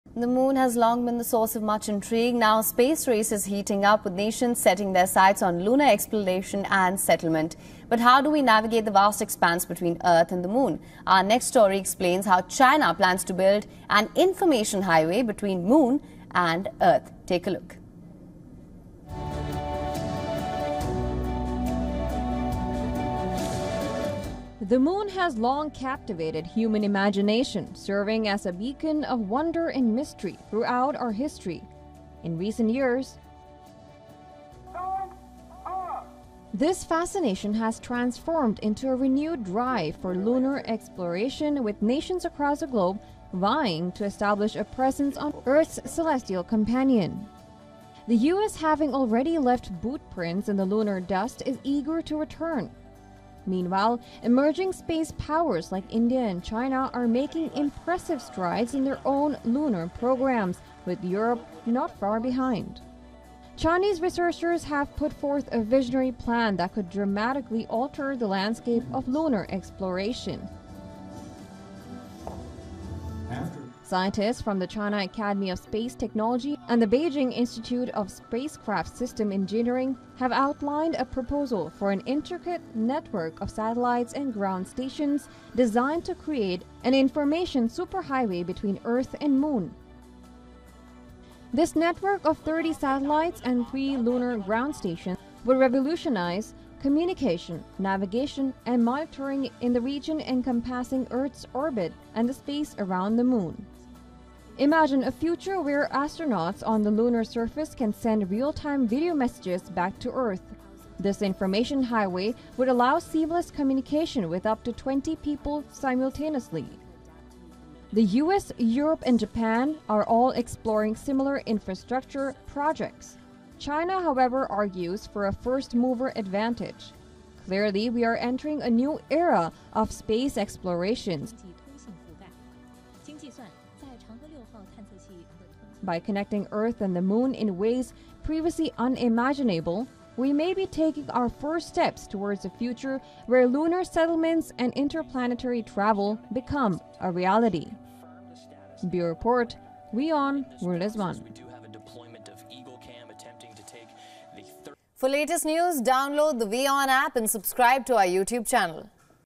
China proposes information superhighway between Earth and Moon | Latest News | WION
Chinese scientists have unveiled a road map to build infrastructure between Earth and the moon to meet national strategic needs and make space travel easier and safer for all. Watch in for more details!